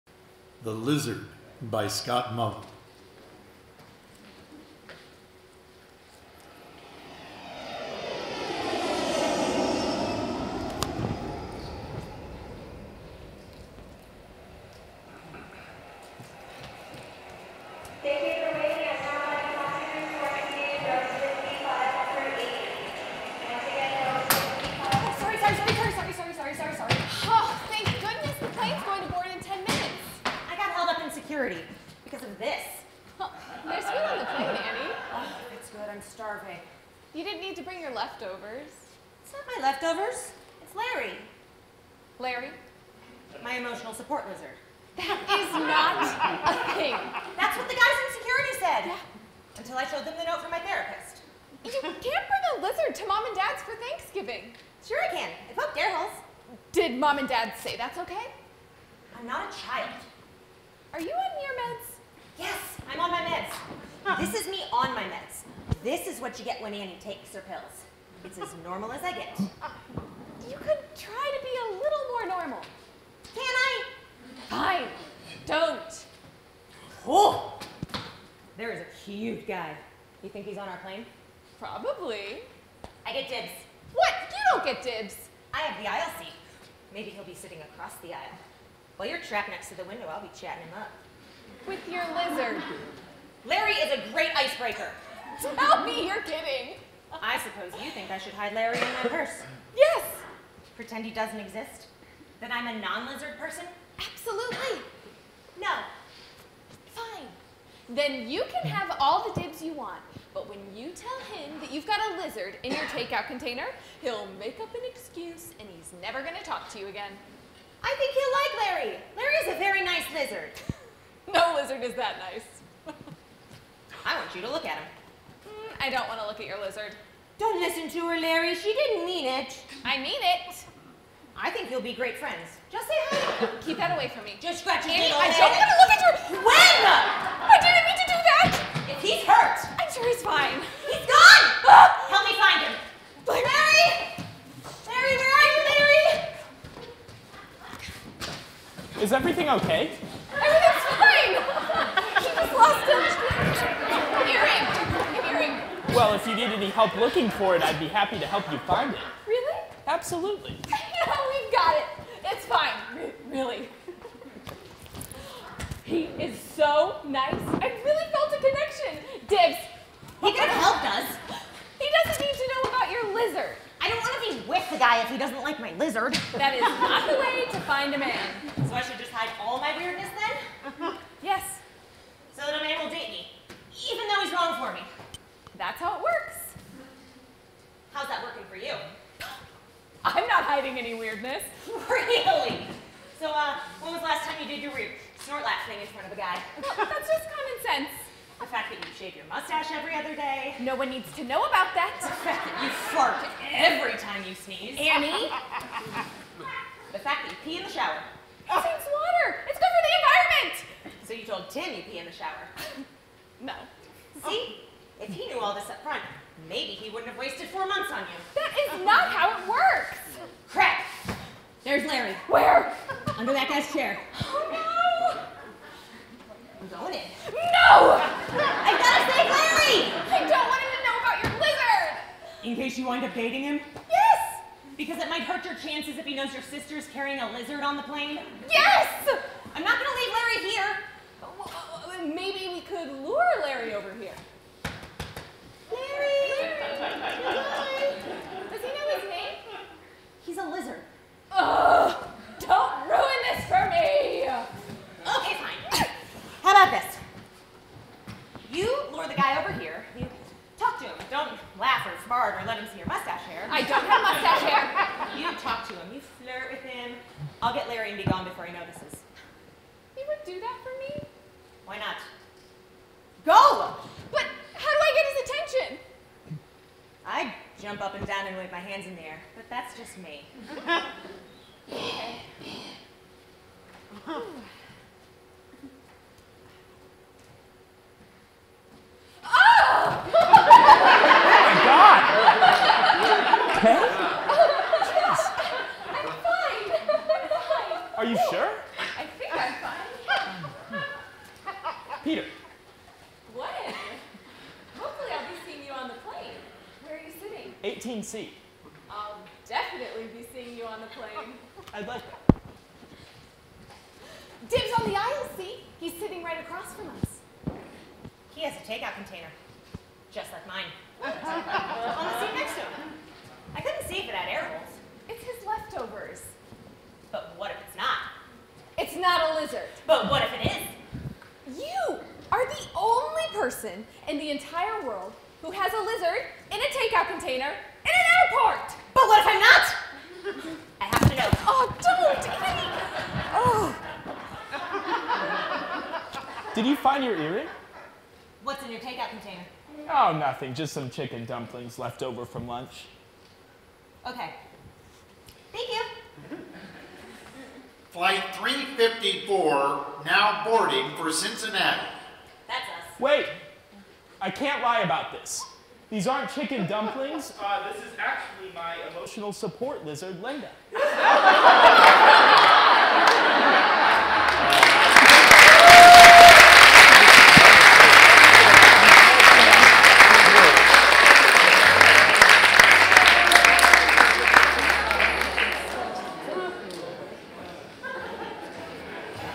The Lizard by Scott Mullen -- 1ST PLACE WINNER!
Directed by Brian Westerley
Featuring Heather Culotta, Lindsay Dunphy and Jay Farrell

Weathervane celebrates the art of the short-format play with our 10th annual 8x10 TheatreFest. These submissions would have been seen during our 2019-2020 season. Each play is written with a specific prop selected by the previous season’s audiences.

Each of the eight 10-minute plays in the festival featured a common item: a take-out food container.